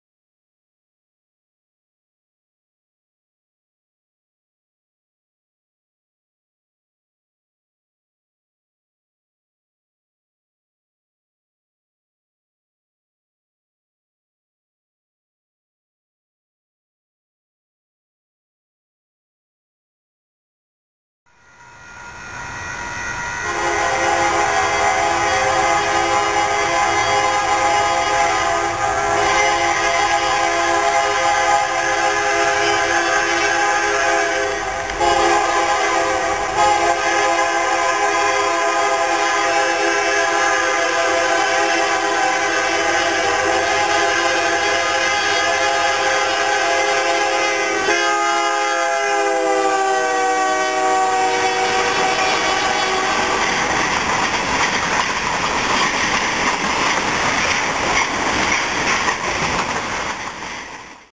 Eastbound Manifest, 09/06/06, Arlington, Texas
This video was taken at about 10:14 PM CDT at the Center St crossing of the UP Dallas Subdivision in downtown Arlington, Texas.  Power for this train was UP 4725 (SD70M) and UP 9106 (C40-8); 4725 also provided the horn.